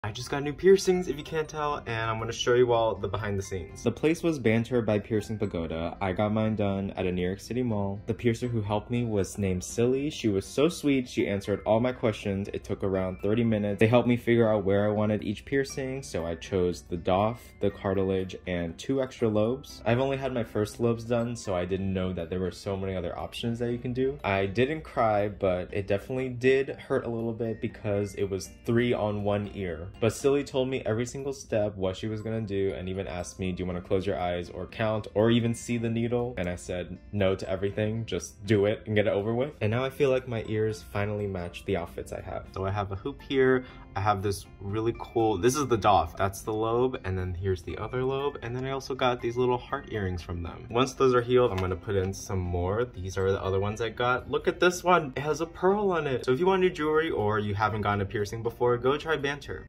I got 4 piercings in one day… 😭 @banterbypiercingpagoda
It's been 3 years since I've got a new piercing and I just got 4 new piercings in one day! 🥲 I’ve always wanted one of my ears to be stacked with piercings because it looks so pretty! Banter has helped me achieve this to become fully decorated in jewelry 💎

The first time I ever got my ears pierced was in College as a dare. My family didn't allow me to get any piercings (they don’t care now), so it felt freeing to do something for myself without asking for permission. And since then, I've received all of the piercings I've wanted and now I get to mix and match my jewelry to create outfits. @banterbypiercingpagoda

Offer valid in stores and online through November 16, 2022. Exclusions apply. Cannot be combined with any other offer.